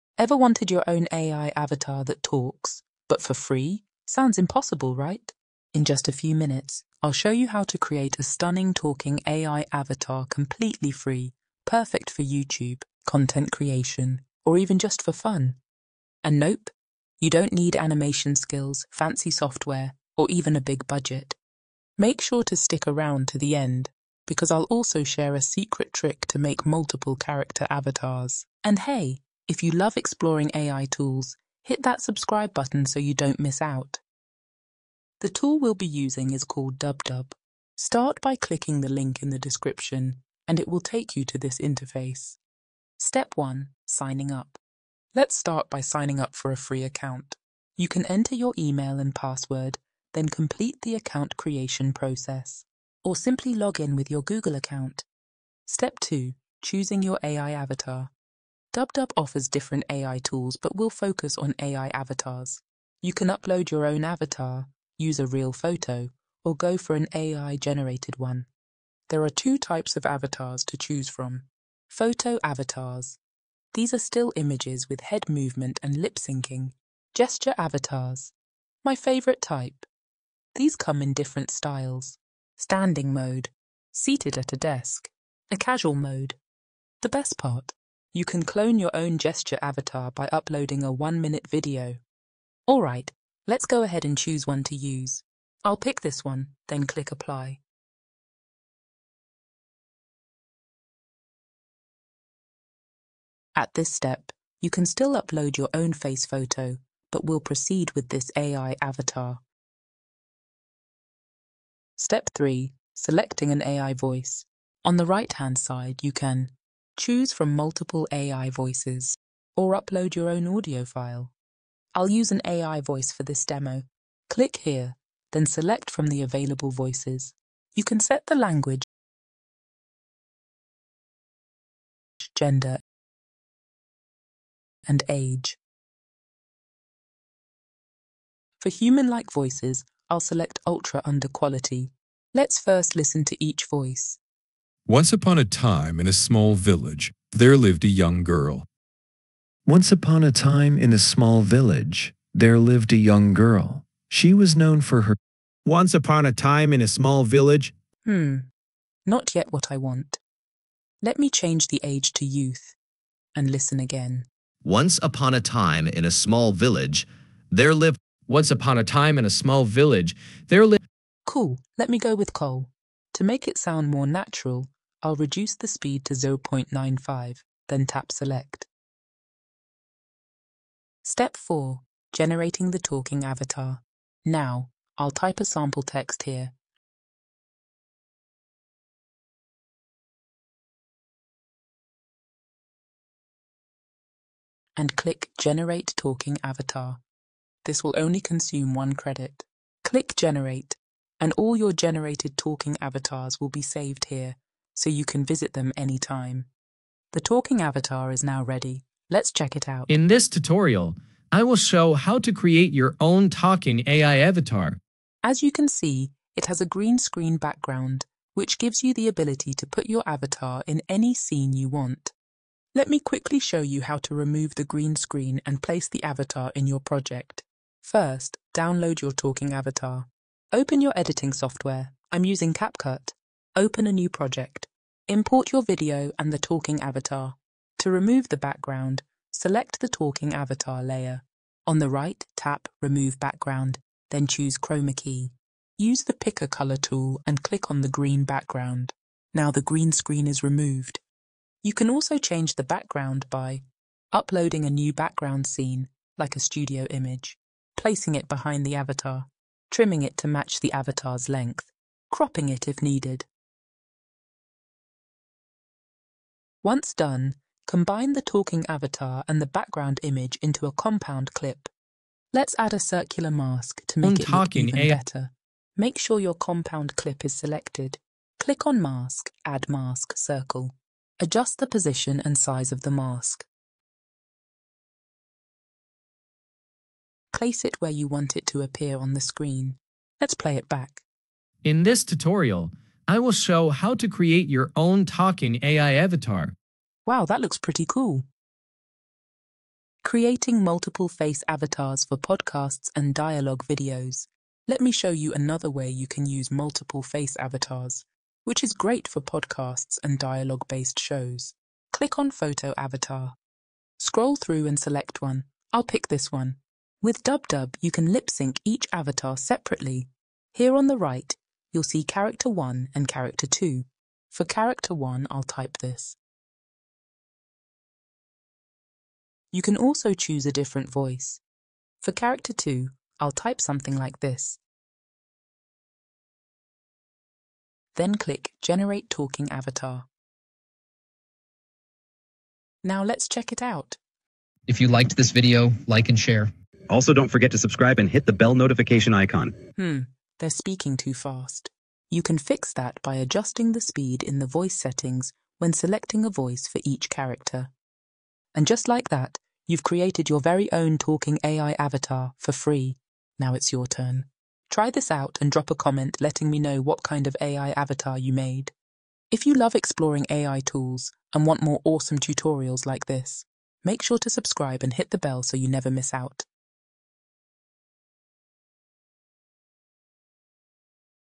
Make a Talking AI Avatar in Minutes (No Cost, No Code!)
Want to create a talking AI avatar for free? In this tutorial, I'll show you how to use DupDub AI to generate a realistic talking avatar with lip-syncing, head movement, and even custom gestures! Plus, I'll guide you through removing the green screen and editing your avatar using CapCut.

🎬 What You'll Learn:
✅ How to sign up for DupDub AI for free.
✅ How to choose & customize an AI avatar (photo & gesture avatars).
✅ How to select the perfect AI voice and adjust speed for a natural tone.
✅ How to generate your talking avatar with just one click.
✅ How to remove the green screen and change the background in CapCut.
✅ How to use multiple avatars for podcasts and dialogue-based videos.